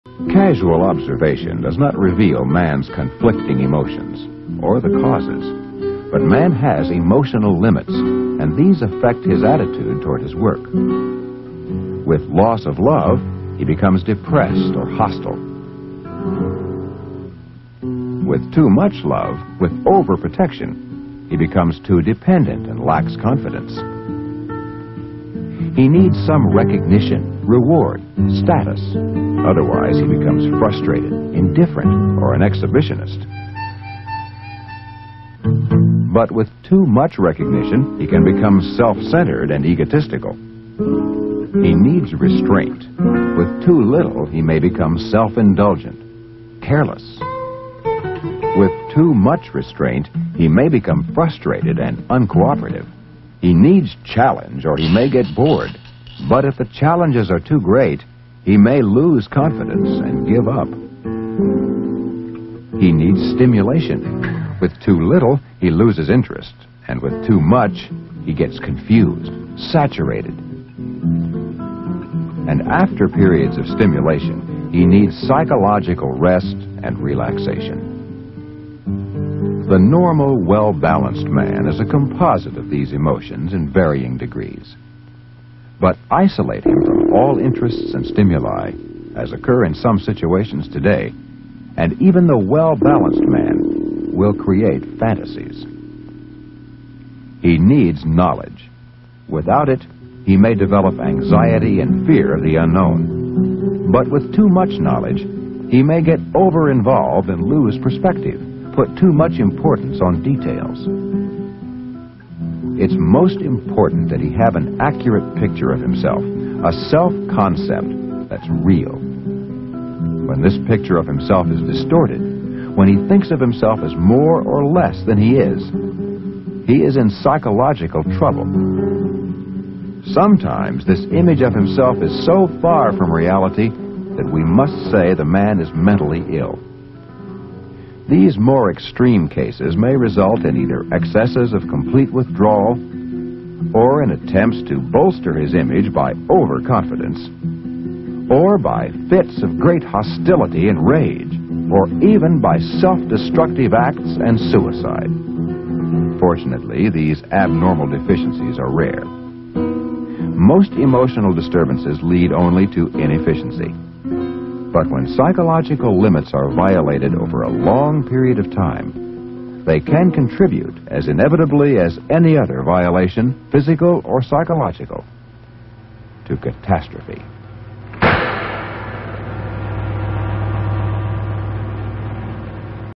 old cartoon psychological trouble
An old informational cartoon about psychological trouble in the workplace.

Seems still relevant.